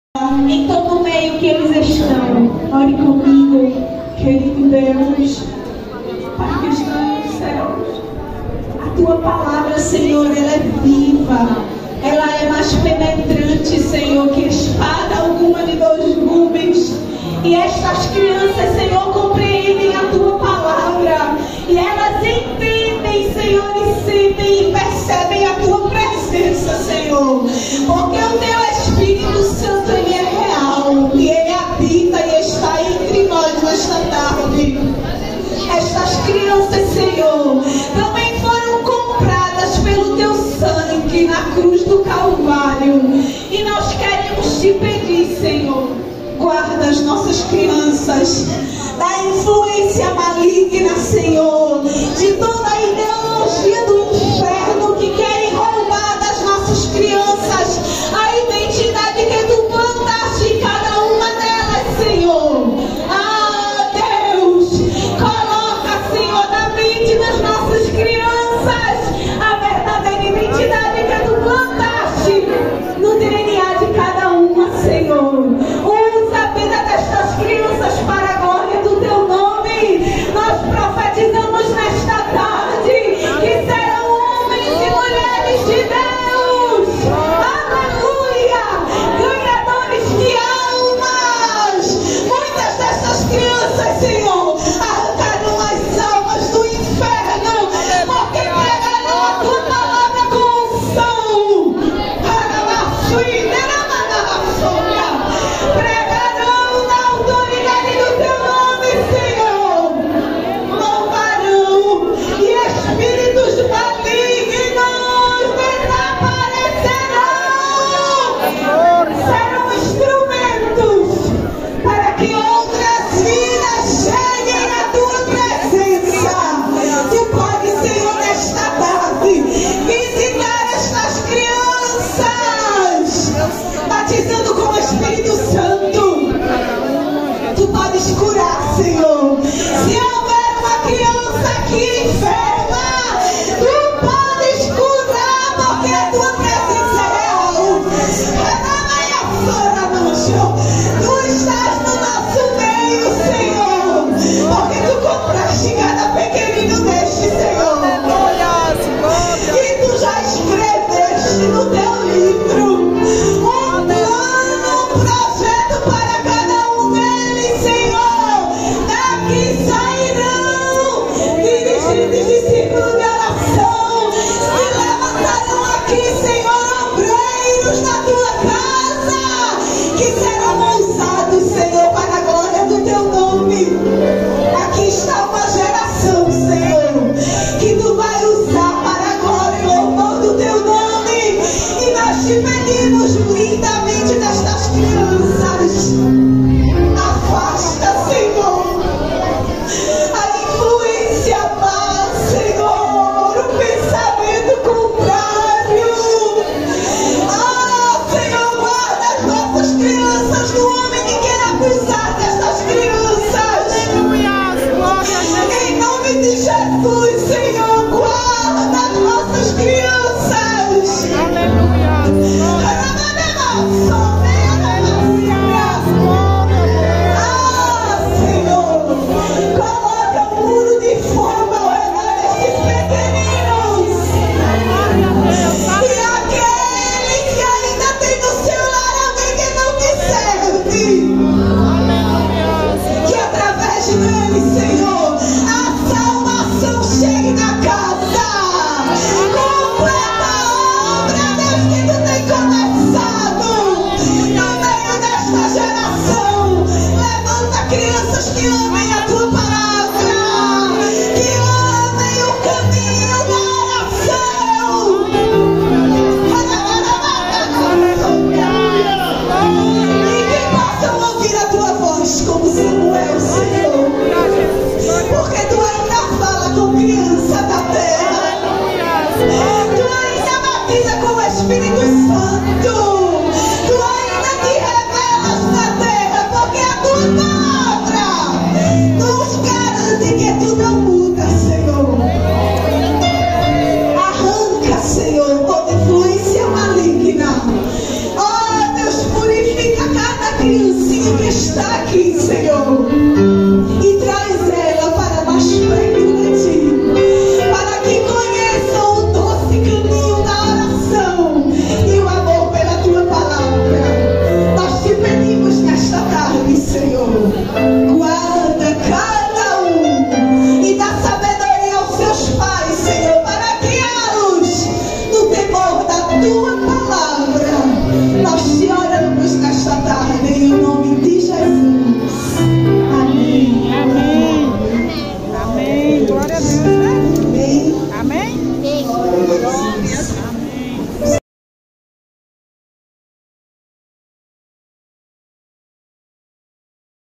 Crianças de joelhos/ oração por todas as crianças - children on their knees
Oi galerinha sejam bem-vindos ao meu cantinho, no vídeo de hoje venho compartilhar com vocês, o momento de oração por todas as crianças do Brasil e do mundo. assista e sejam abençoados. curta, compartilhar para outras pessoas serem abençoadas.agradeço seu carinho e que papai do céu abençoe vocês.